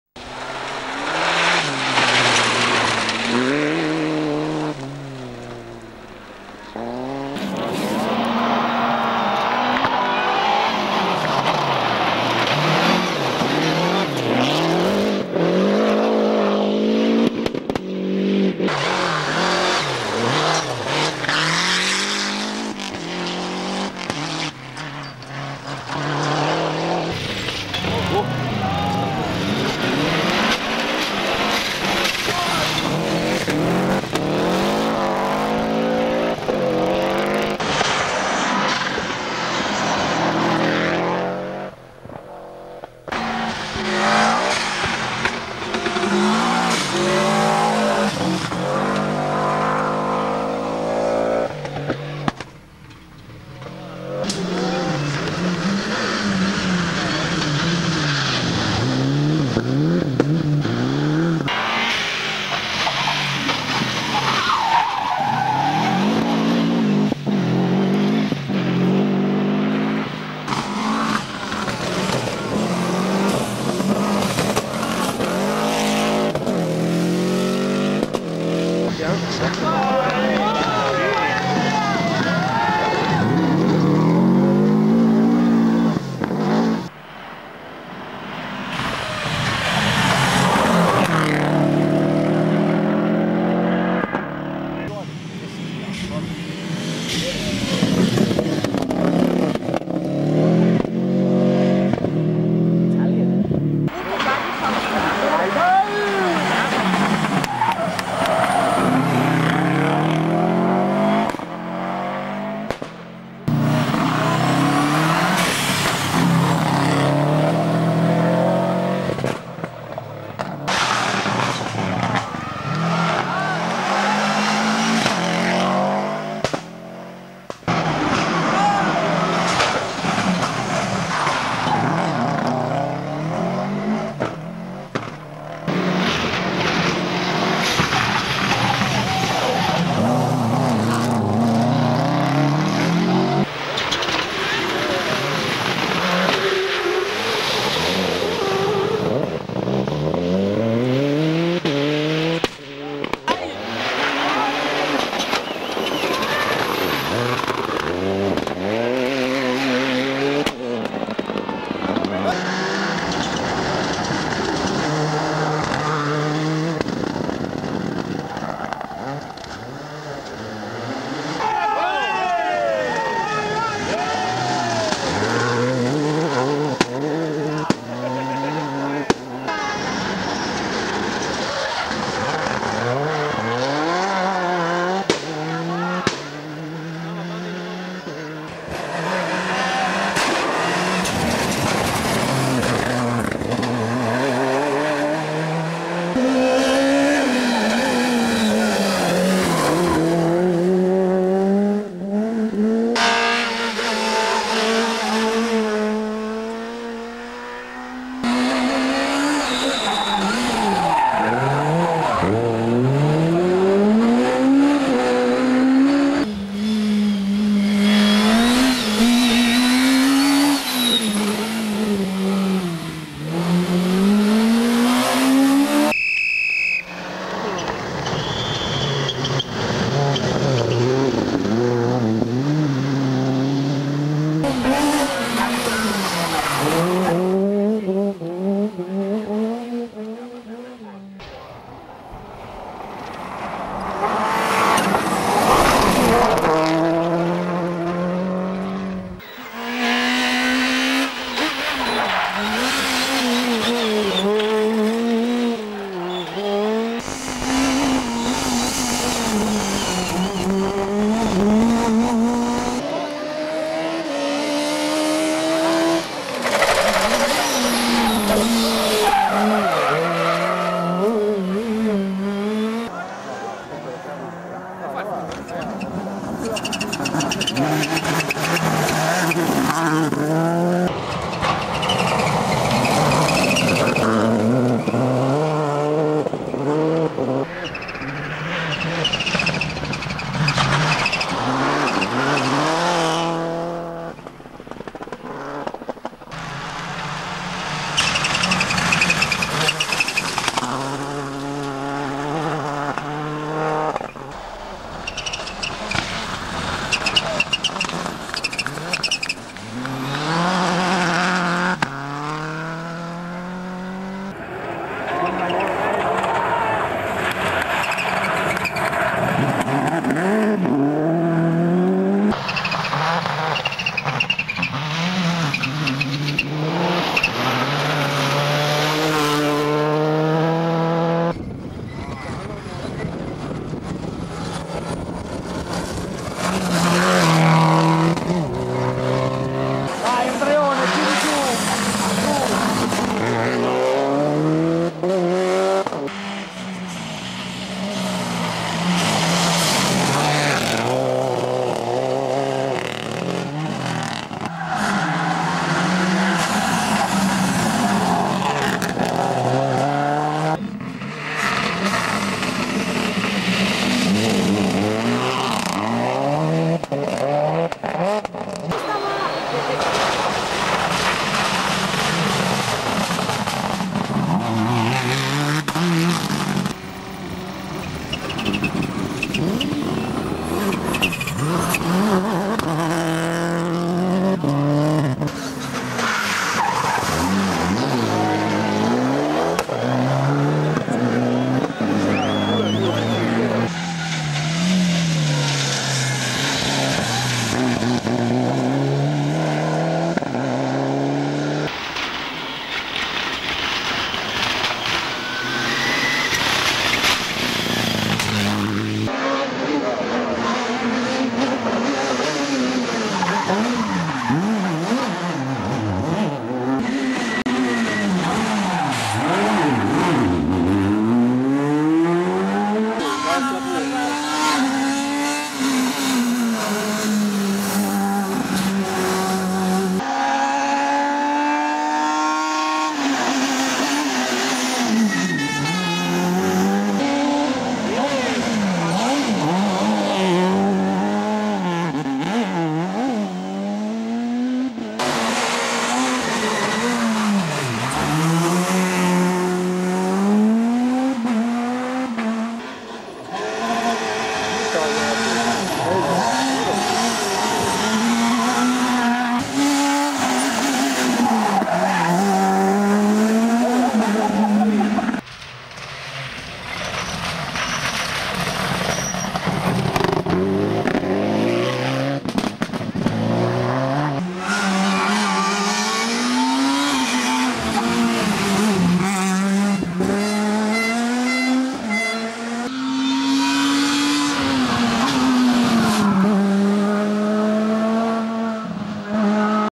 Andrea Navarra Tribute
Tributo al grande pilota cesenate Andrea Navarra che ha emozionato gli appassionati di rally nel periodo a cavallo tra gli anni 90 e l'inizio 2000.